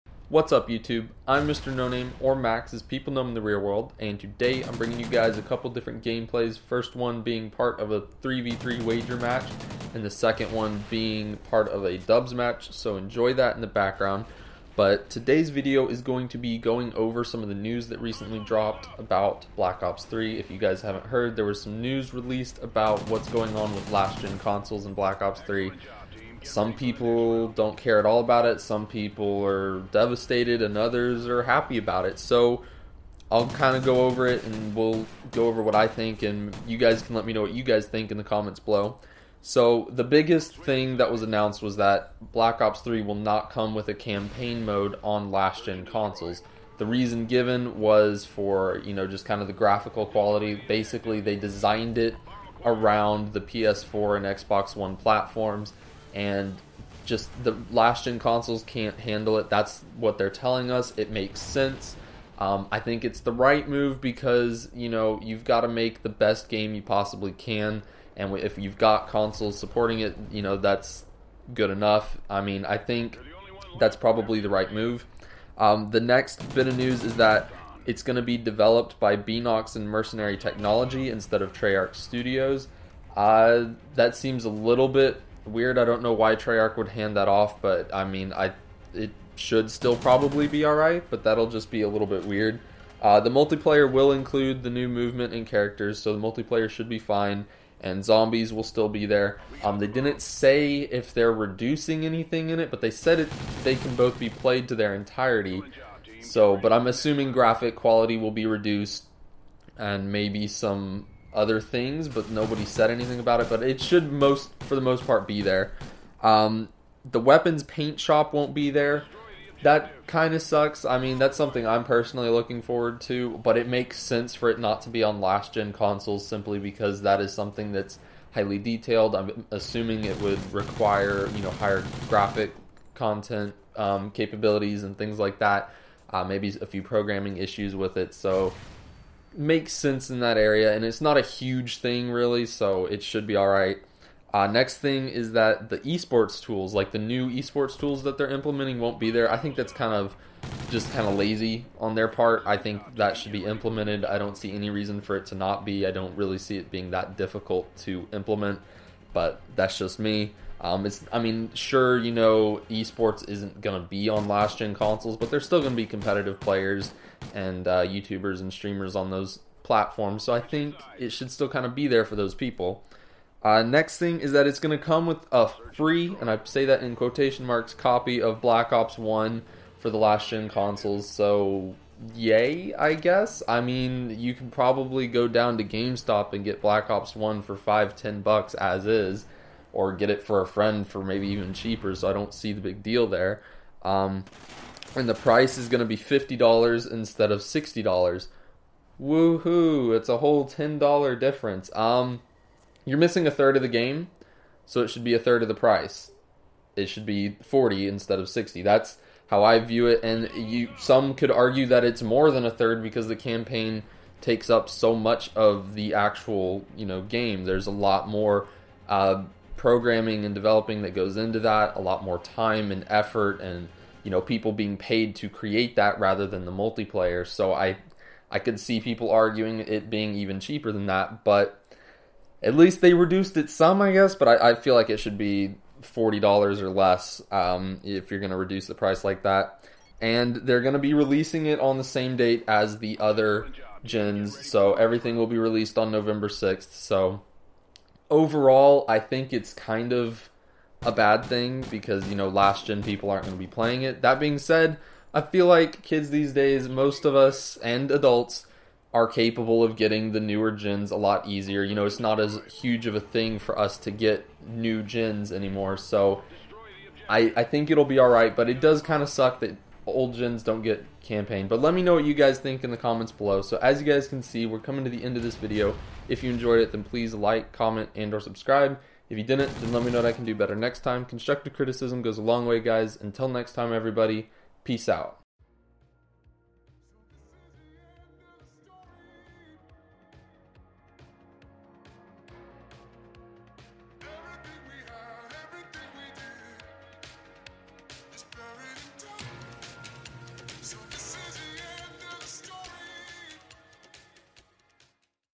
BO3 News: Sorry Last Gen Players, No Black Ops 3 Campaign For You!
Competitive Call of Duty Ghosts wager and umg gameplays. In this video, I discuss the most recent news about black ops 3 on last gen consoles (ps3 and xbox 360).

Partner Network: TGN

Profile Pic, Outro, & Header Done By: Jebby